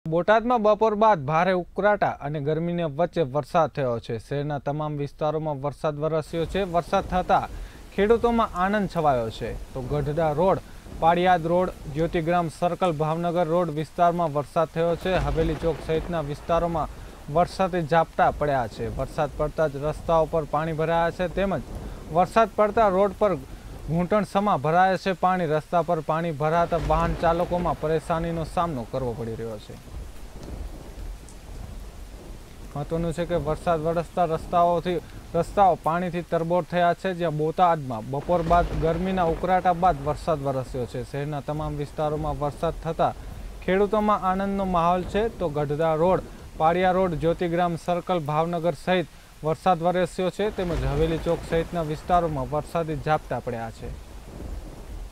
બોટાદ: બપોર બાદ ભારે ઉકળાટ અને ગરમી વચ્ચે વરસાદ
શહેરનાં તમામ વિસ્તારોમા વરસાદ થતાં ખેડૂતોમાં આનંદ છવાયો
ગઢડા રોડ, પાળીયાદ રોડ, જ્યોતિગ્રામ સર્કલ, ભાવનગર રોડ વિસ્તારમાં વરસાદ
હવેલી ચોક સહિતનાં વિસ્તારમાં ઝાપટા સાથે વરસાદ
વરસાદ પડતા રસ્તાઓ પર ભરાયા પાણી
વરસાદ પડતા રોડ પર ઘૂંટણ સમા ભરાયા પાણી
રસ્તા પર પાણી ભરાતા વાહન ચાલકો પરેશાન